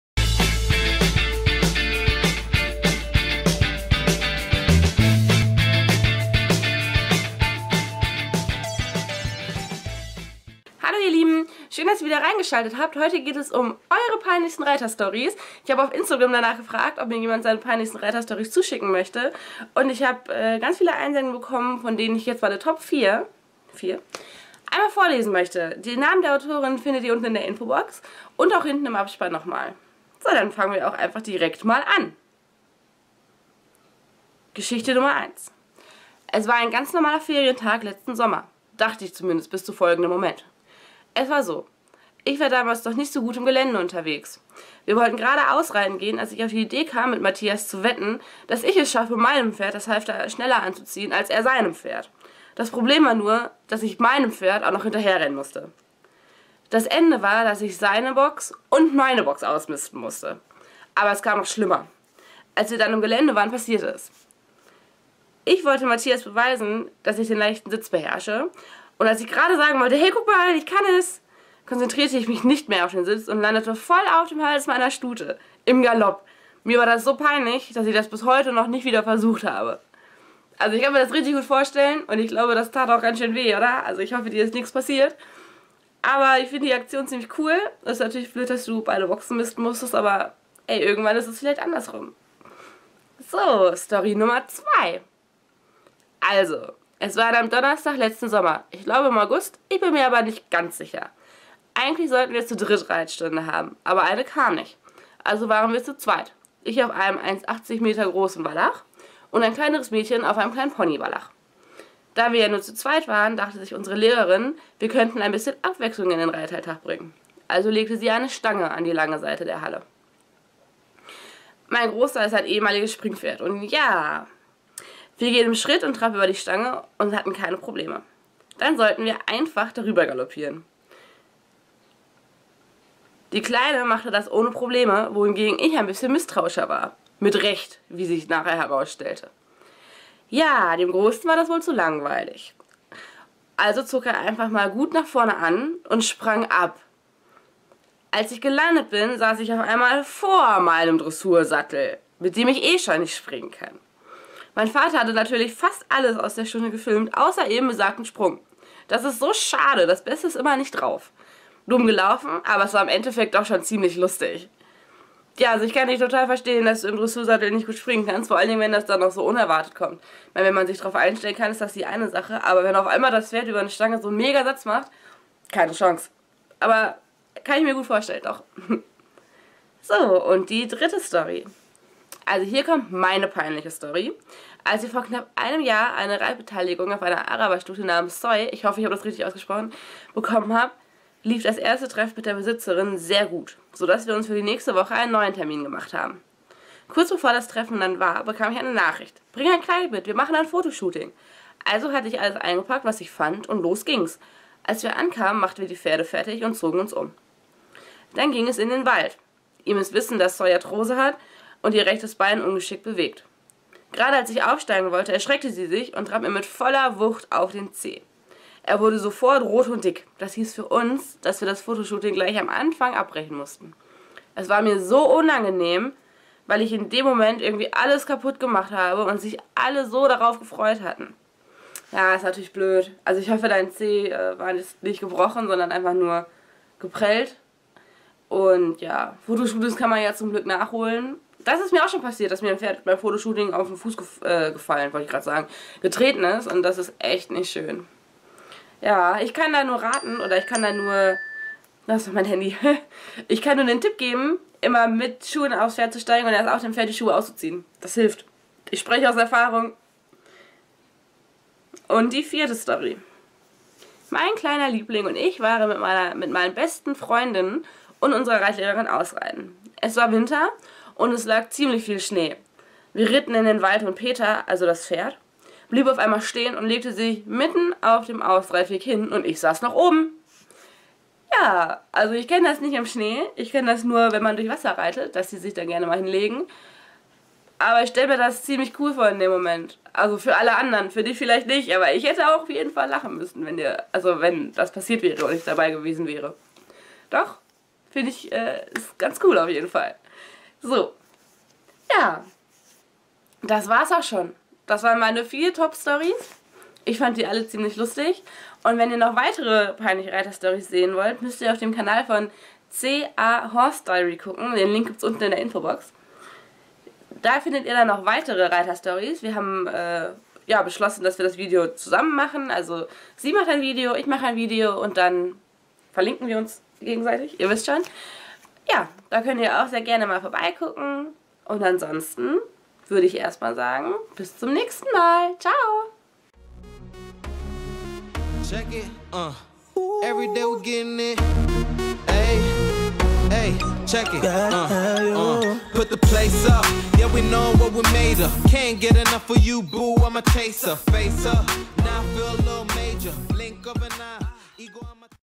Eure peinlichen Reiterstories
Hier habe ich n un 4 eurer peinlichsten Reiterstories zusammengefasst. Guckt für den zweiten Teil doch auch mal bei CA Horsediary vorbei

... ask.fm/fraeulein_pusteblume